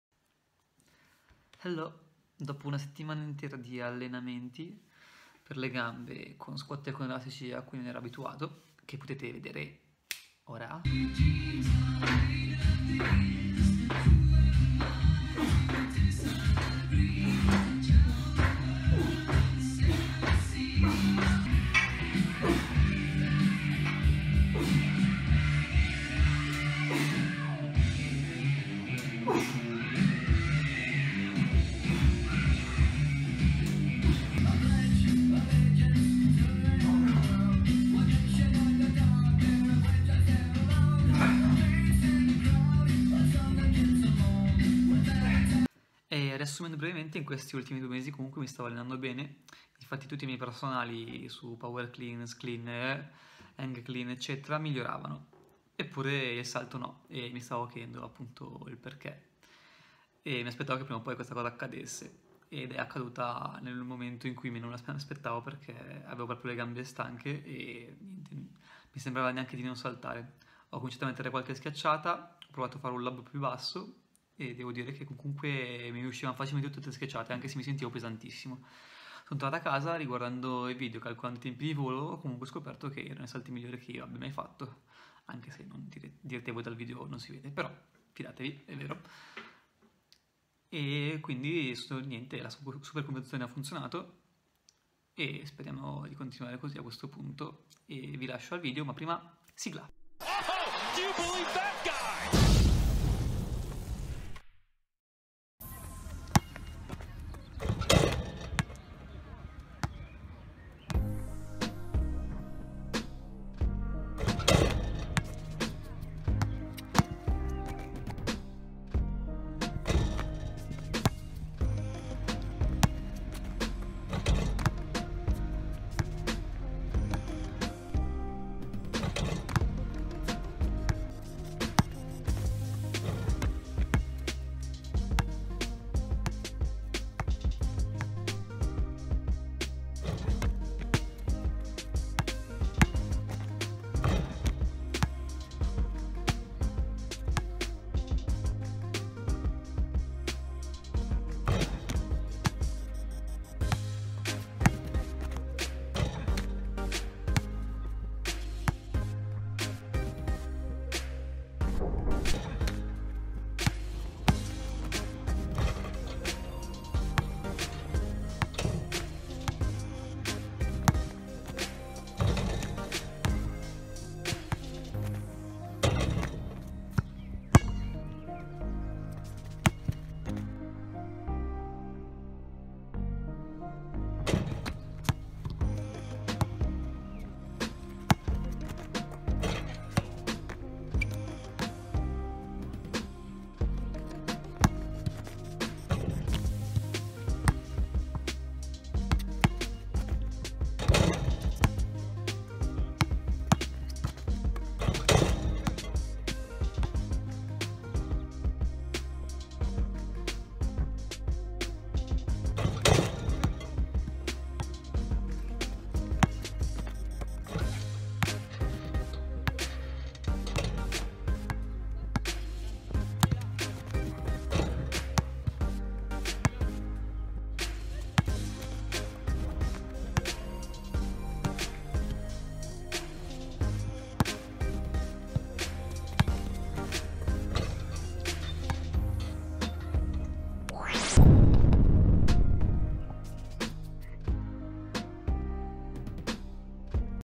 Supercompensation WORKS! 5'7 Dunker #1
finally the supercompensation work i i reached my peak for now,  i'm sure that i have more inch in box